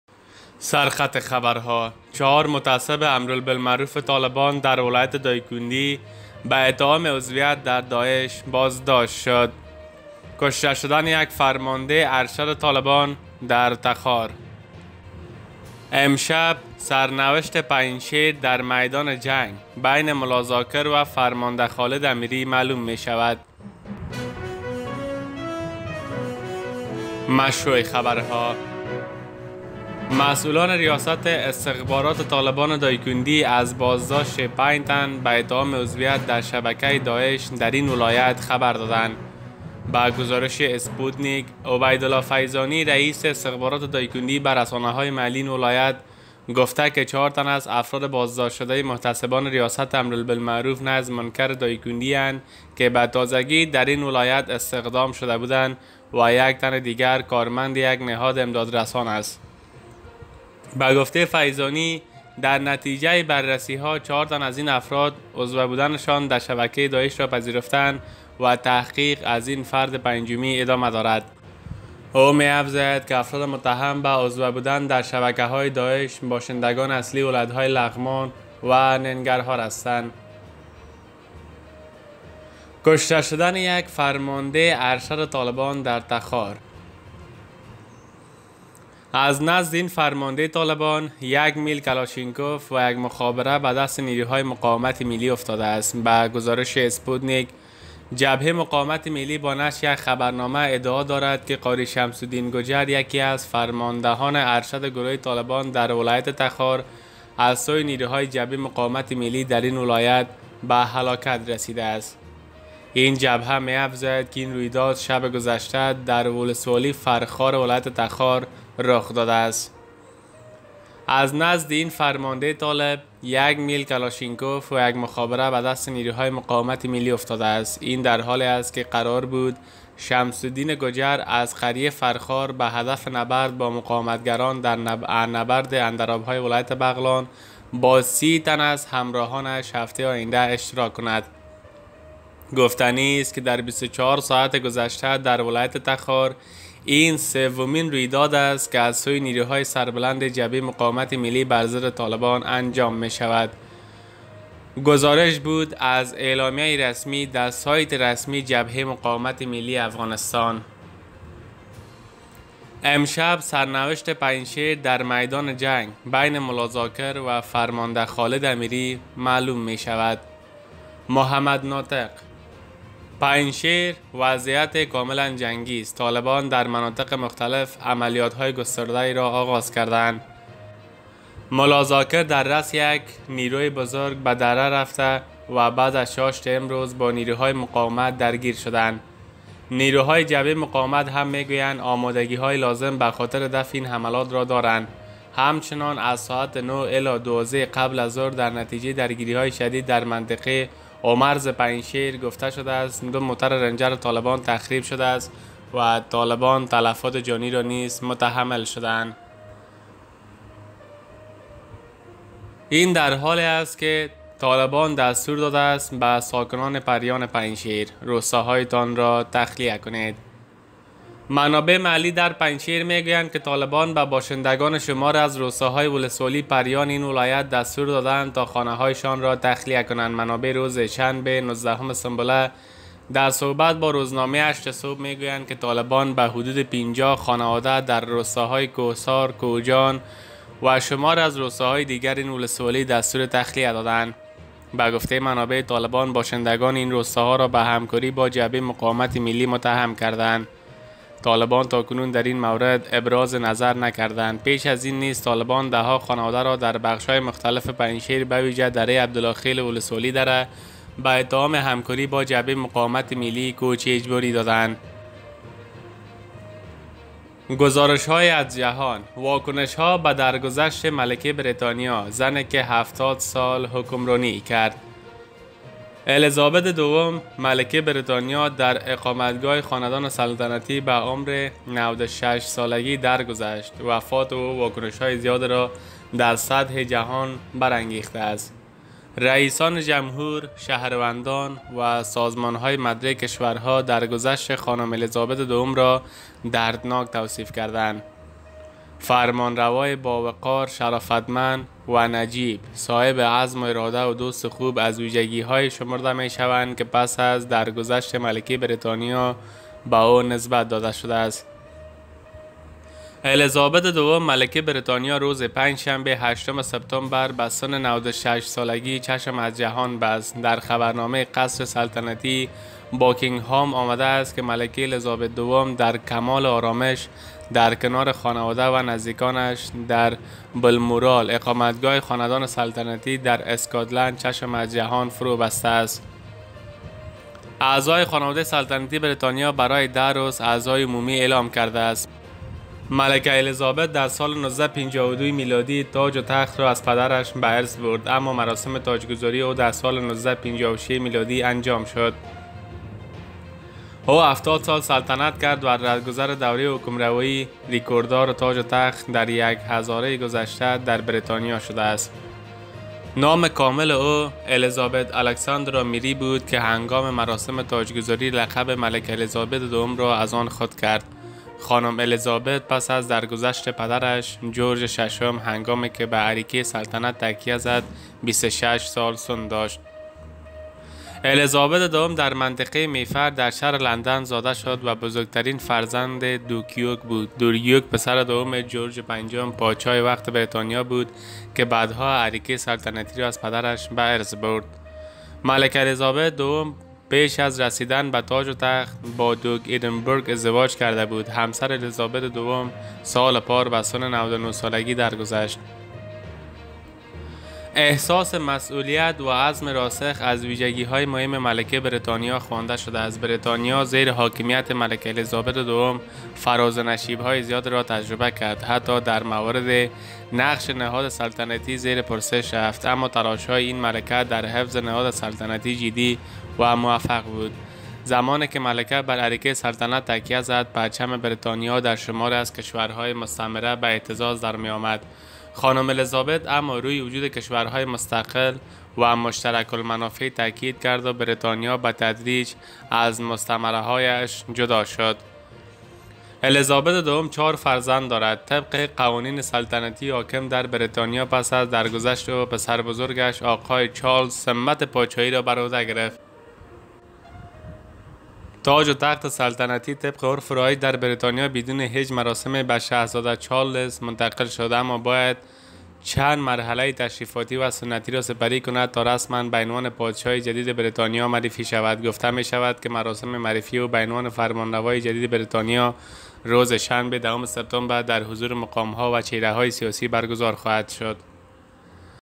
امشب پو.ست و گوشت ملا. ذاکر در امبولانس مُرده کشی به ملا .یعقوب پارسل شد
مهم اخبار خبرهای تازه